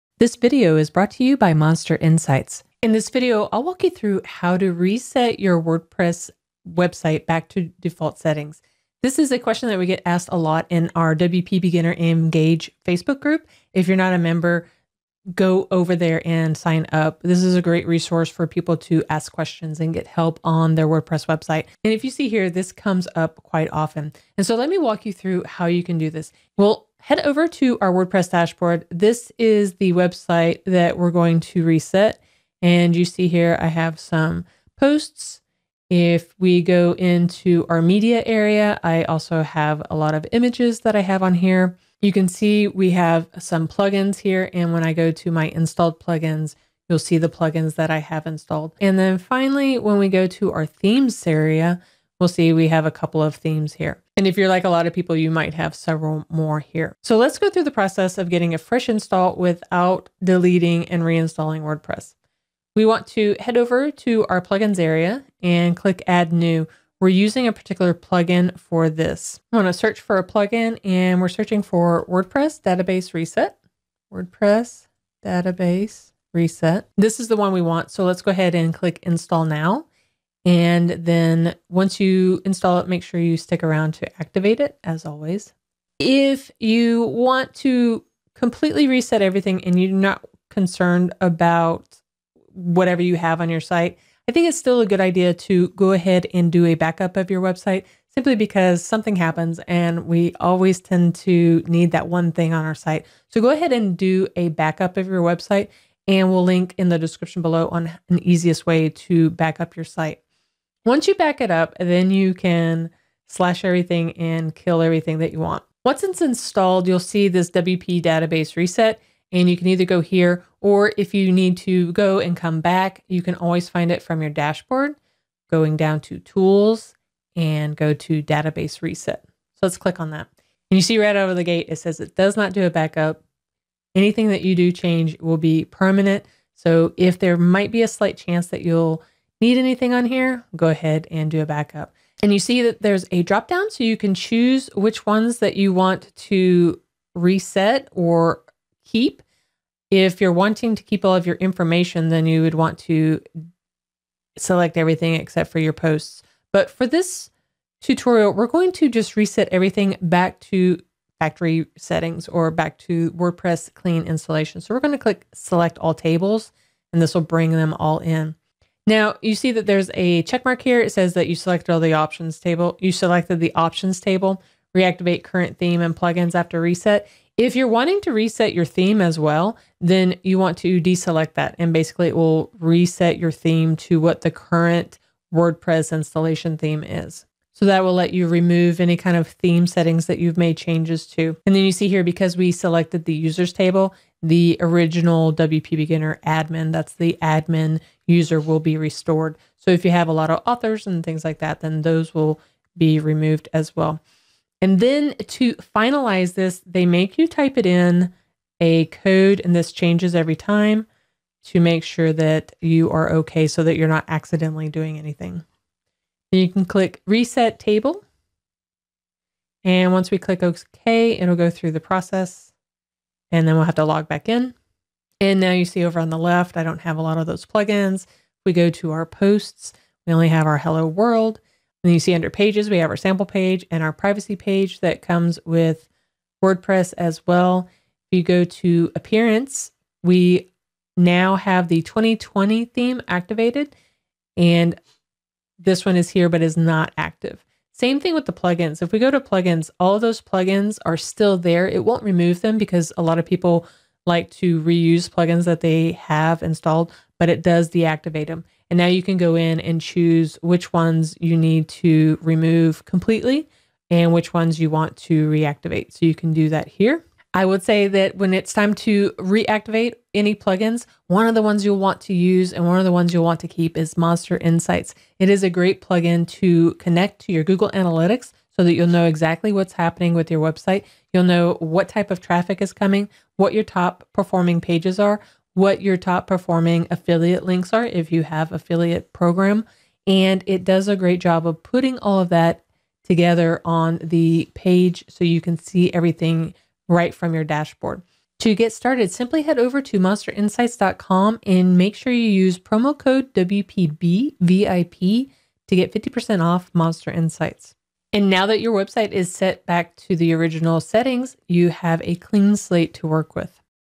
How to Reset Your WordPress Database to Default Settings
Have you found a need to reset your WordPress site to the default settings? In this video, we will show you how to reset your WordPress database to the default settings.

For the discount, use our code: WPBVIP

For this tutorial we will be using the WordPress Database Reset plugin found here:

Before resetting your content, we would recommend creating a backup for safety. With the plugin installed and activated, you would go under Tools, Database Reset to choose what tables to reset. You can select specific tables to reset or the plugin gives the option to select all tables.

You can decide to reactivate your plugins and theme or not depending on your preference. With that selected, you will add the security code and press submit. The plugin will give you a final warning and as soon as you hit the Okay button it will begin the process of removing your content.